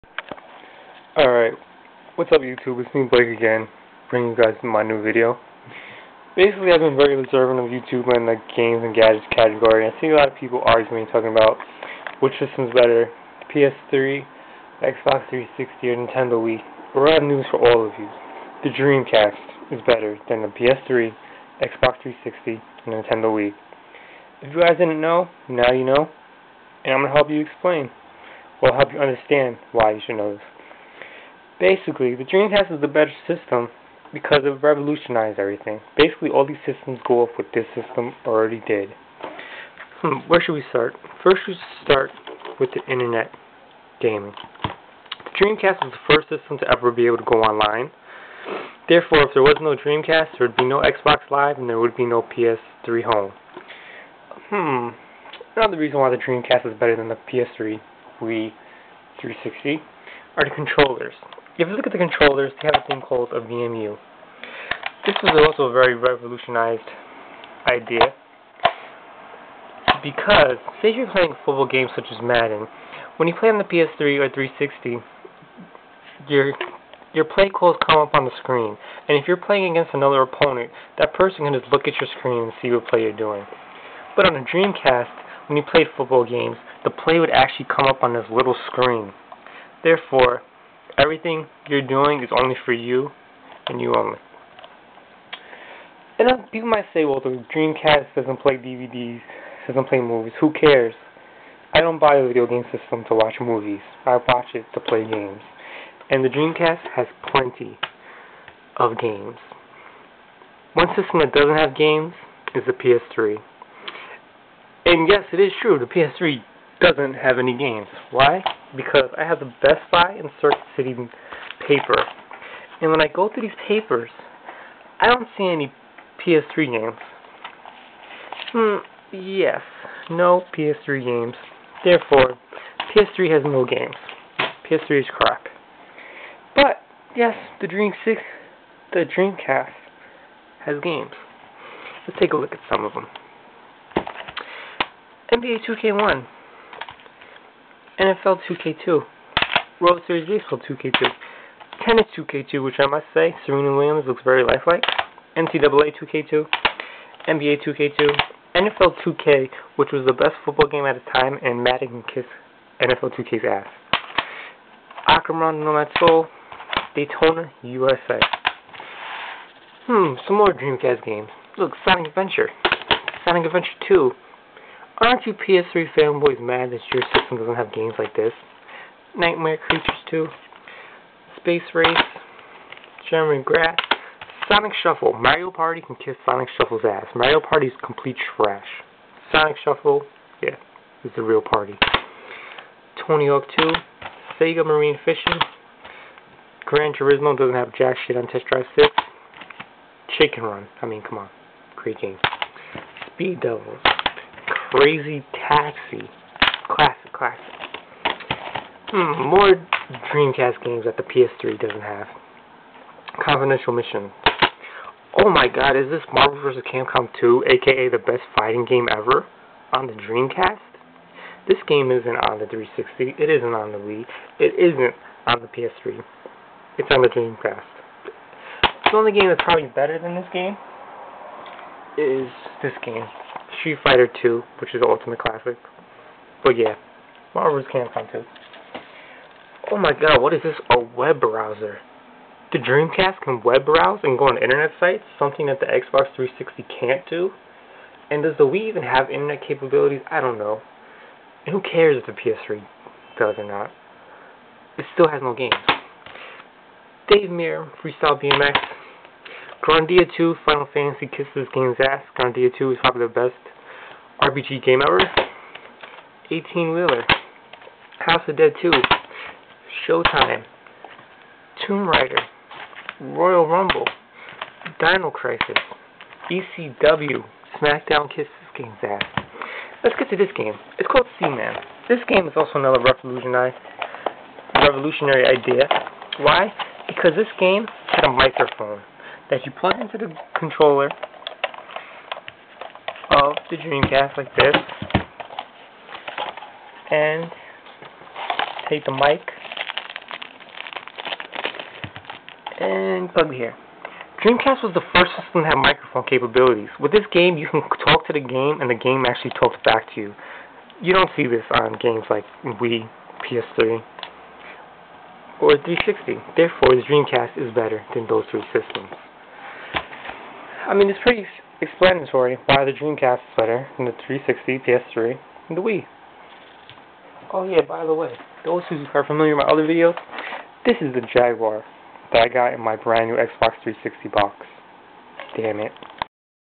Why The DREAMCAST is better than the PS3, Wii, and Xbox 360
this is my video explaing why the SEGA Dreamcast is better then the nintendo Wii, PS3 and the Xbox 360